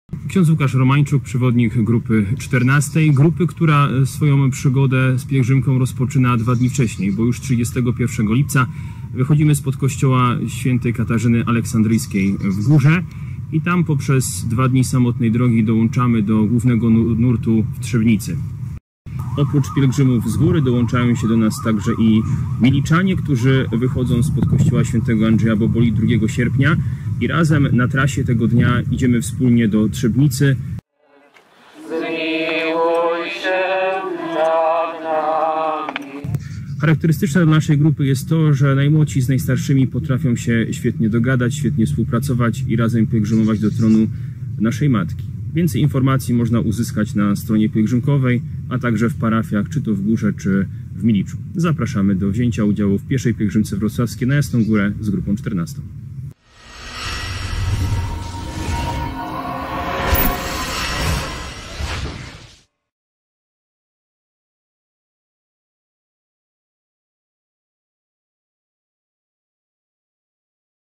Z kim na PPW2019?
Zbliża się Piesza Pielgrzymka Wrocławska. Wybraliście już grupę? A może Grupa 14? Atut? Idą dwa dni dłużej! Zaprasza ks. Łukasz Romańczuk